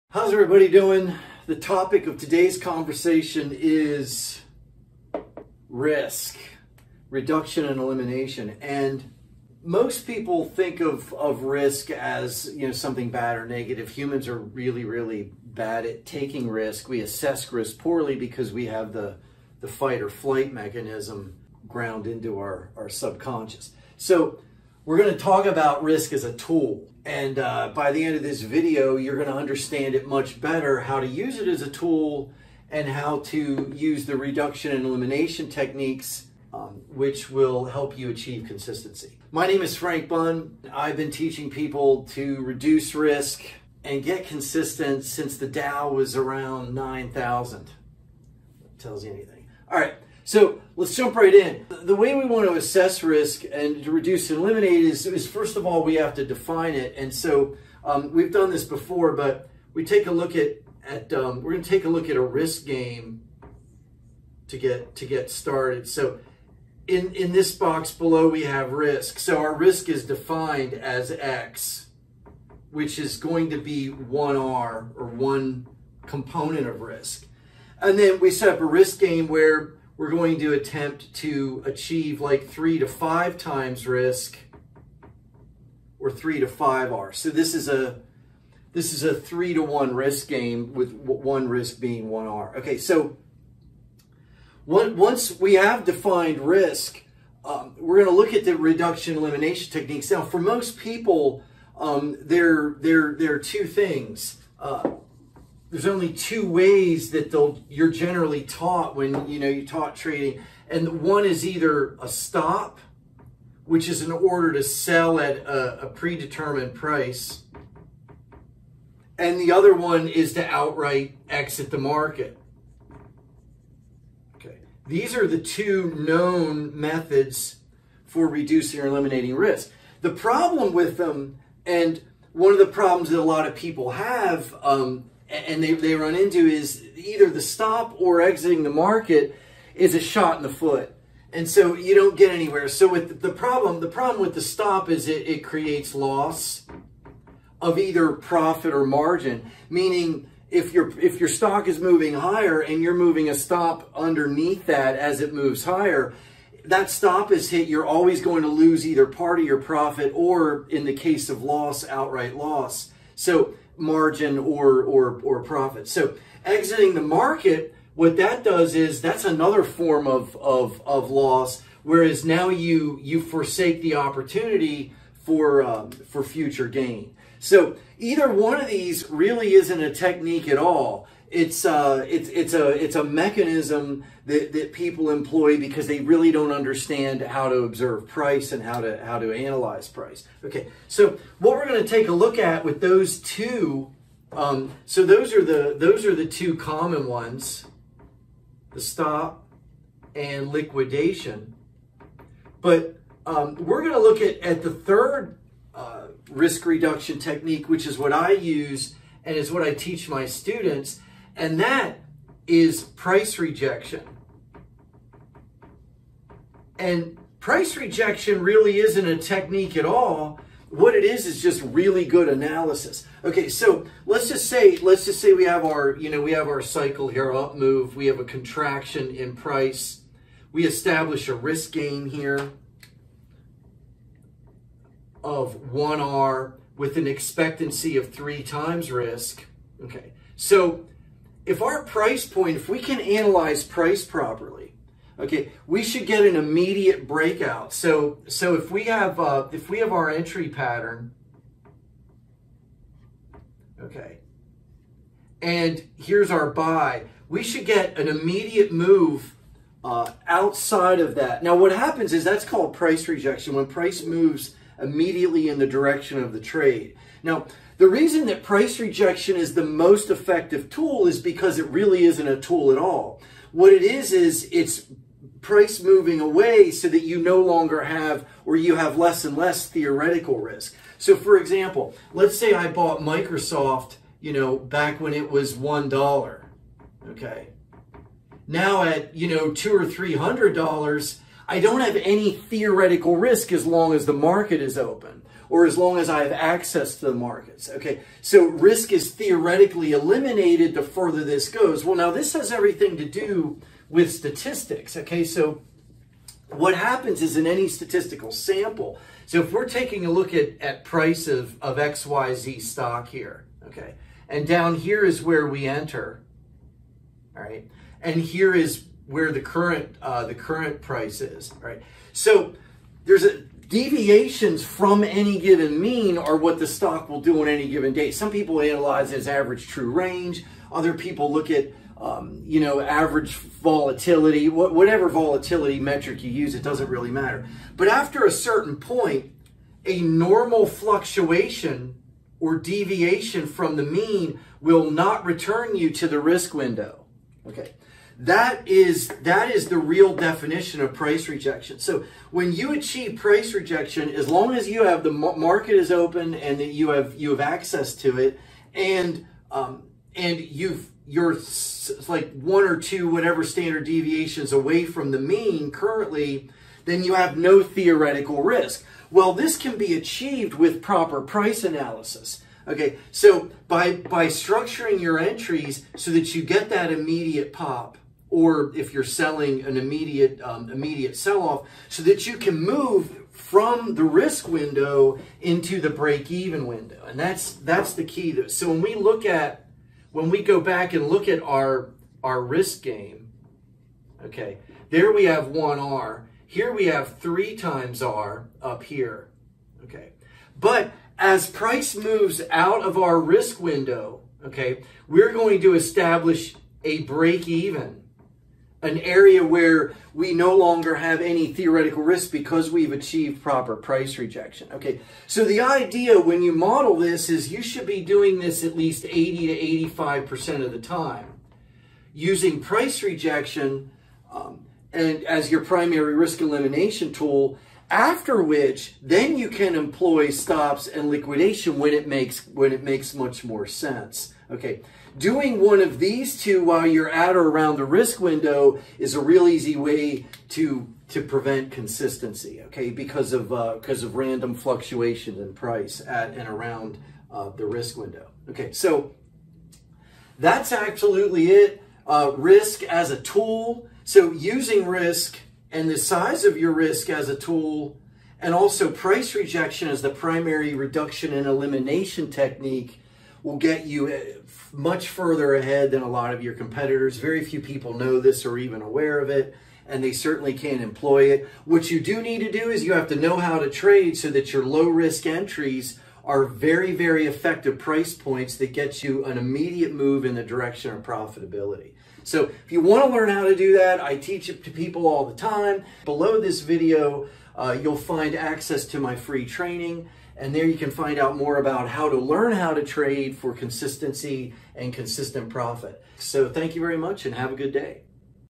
How to use risk as a tool to become profitable
In this video, I'll show you how to use price rejection as the primary reduction and elimination technique to achieve consistency 📈

FREE TRAINING: Learn what it takes to get on the road of consistency. Link to save your spot .